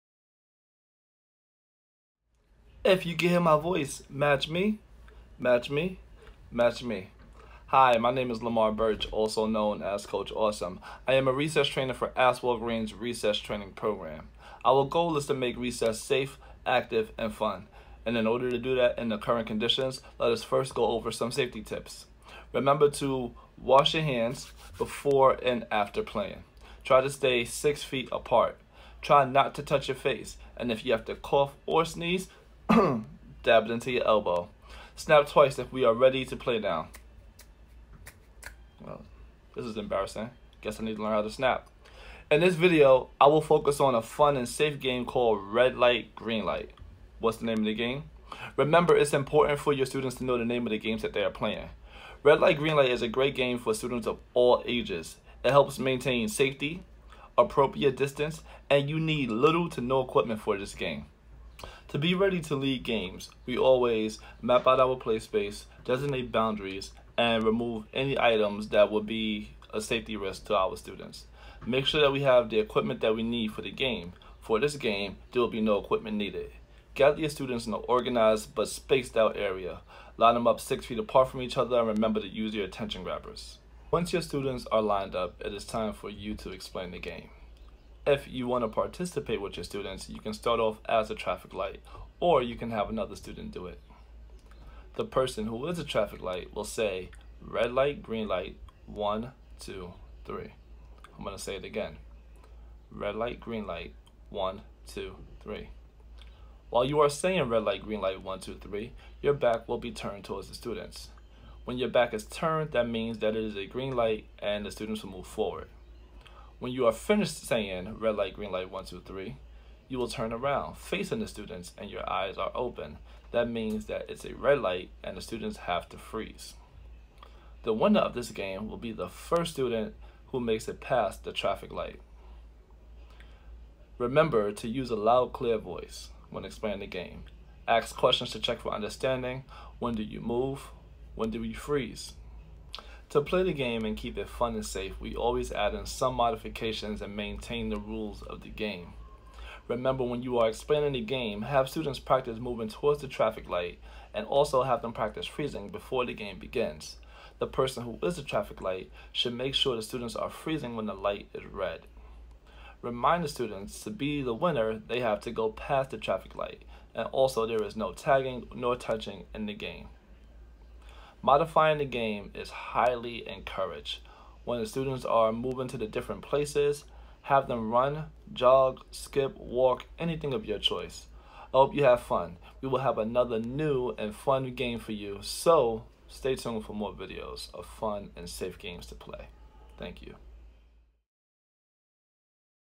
Red Light, Green Light
Red Light, Green Light is the perfect game to get kids in kindergarten through fifth grade moving and can be modified for any space.